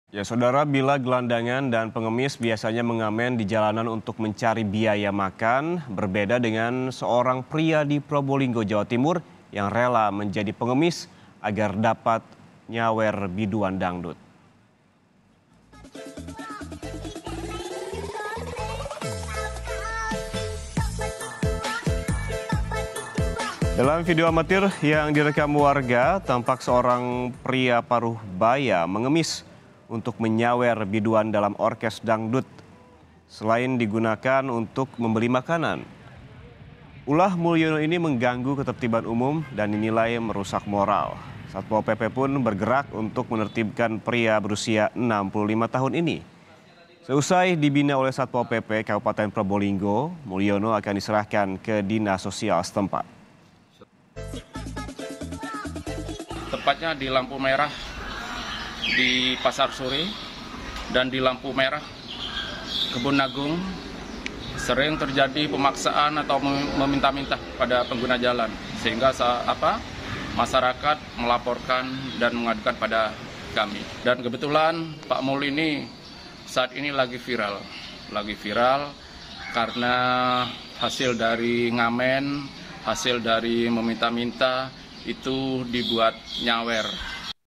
Pria Asal Probolinggo Jadi Pengemis Supaya Bisa 'Nyawer' Biduan Orkes Dangdut
PROBOLINGGO, KOMPAS.TV - Bila gelandangan dan pengemis biasanya mengamen di jalanan untuk mencari biaya makan, berbeda dengan seorang pria di Probolinggo, Jawa Timur, yang rela menjadi pengemis agar bisa 'nyawer' biduan dangdut.

Dalam video amatir yang direkam warga, tampak seorang pria paruh baya mengemis untuk menyawer biduan dalam orkes dangdut, selain digunakan untuk membeli makanan.

Ulah Mulyono ini mengganggu ketertiban umum dan dinilai merusak moral.

Satpol PP pun bergerak, untuk menertibkan pria berusia 65 tahun ini.

Usai dibina oleh Satpol PP Kabupaten Probolinggo, Mulyono akan diserahkan ke Dinas Sosial setempat.